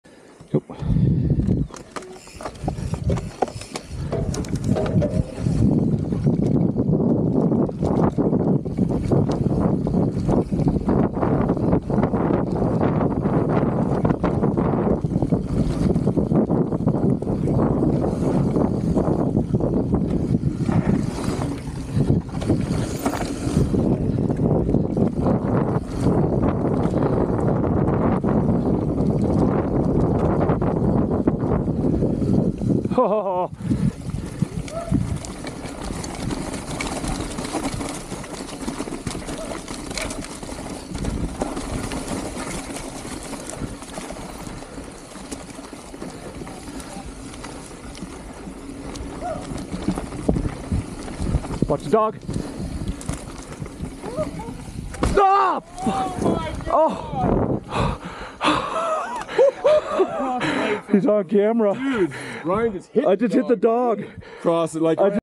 Huallarcocha Freeride with Doggo! - Peru Mountain Biking / Sacred Valley Downhill
January 10, 2024 - Huallarcocha Freeride line with on-trail hazard!  @cuscokintimtbing25 @WanderingWheelsRevelstoke

PS - The dog was okay and actually we saw the owner and dog again later in the trip in the same place and the dog was chasing us again.